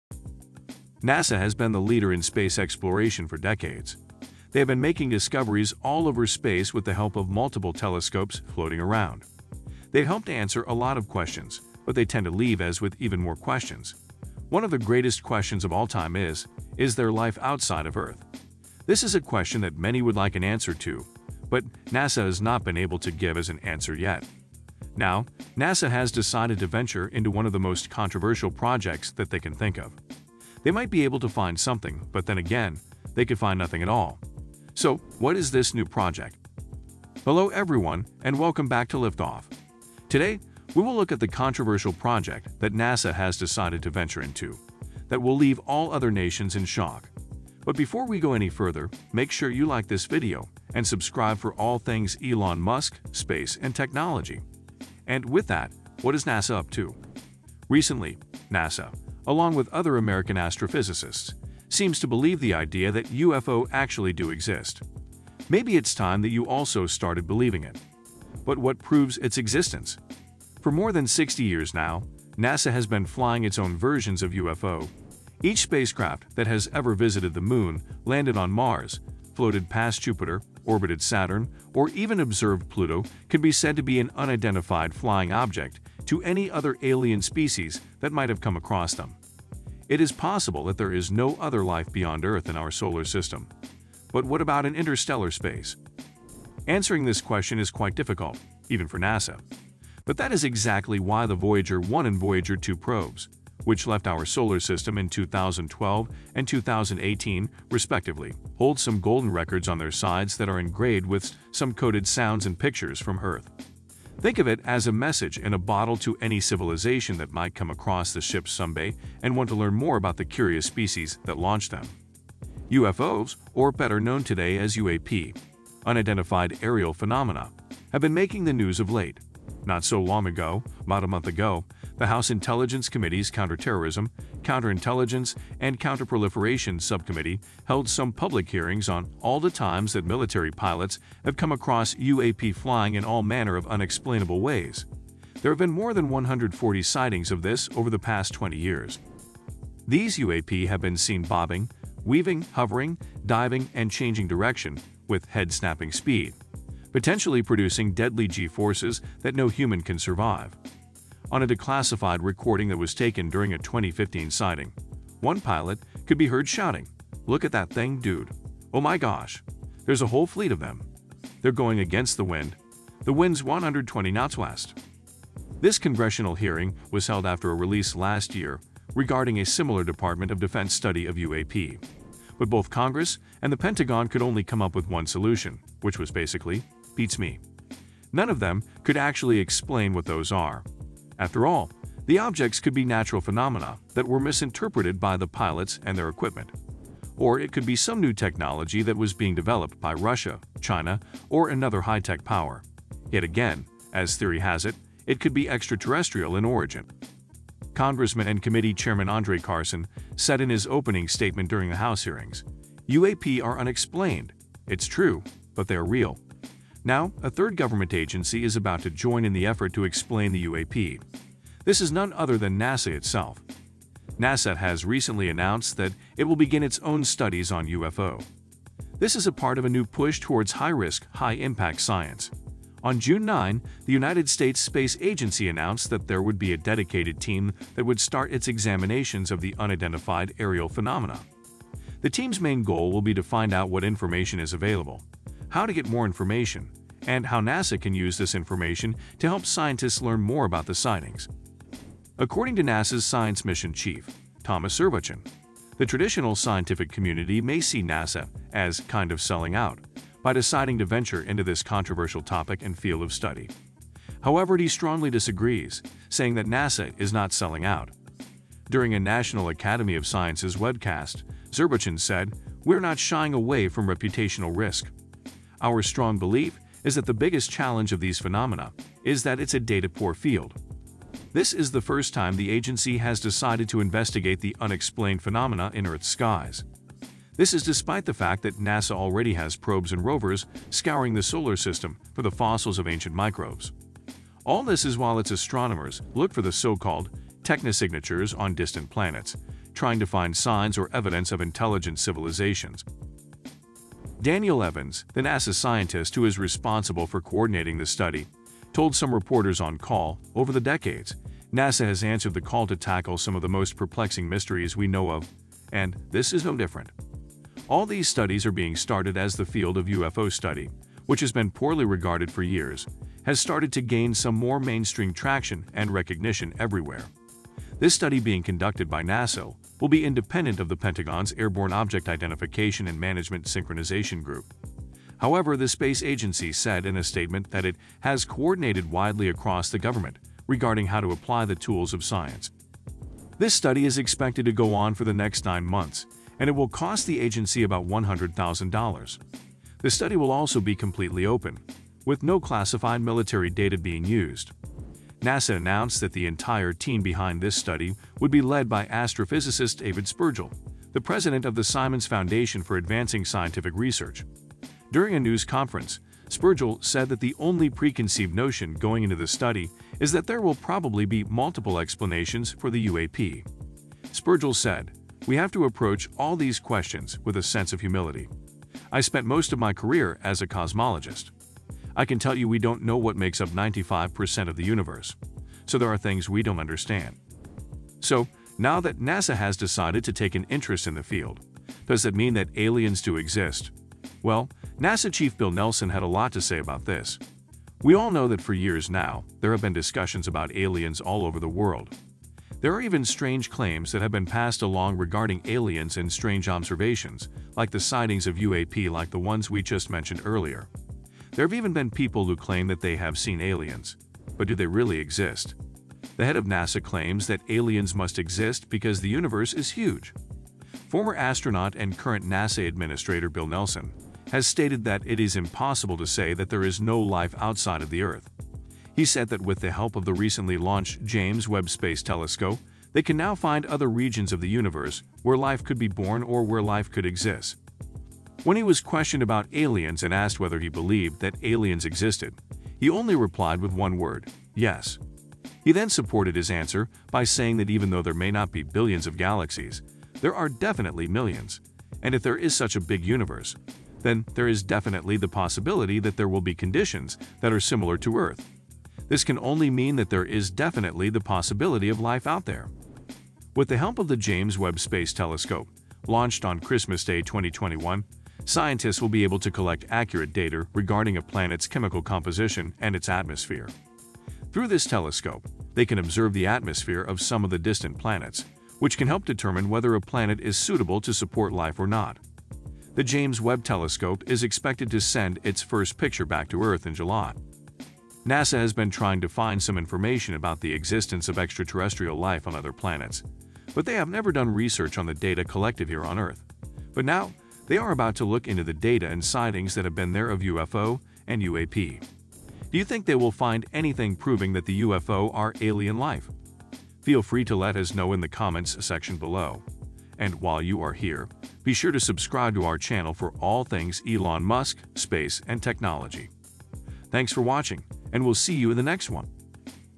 NASA Just Launched This Controversial Project That Will Shock All Nations!
NASA has been the leader in space exploration for decades. They have been making discoveries all over space with the help of multiple telescopes floating around. They have helped answer a lot of questions, but they tend to leave us with even more questions. One of the greatest questions of all time is, "Is there life outside of Earth?" This is a question that many would like an answer to, but NASA has not been able to give us an answer yet. Now, NASA has decided to venture into one of the most controversial projects that they can think of. They might be able to find something, but then again, they could find nothing at all. So, what is this new project?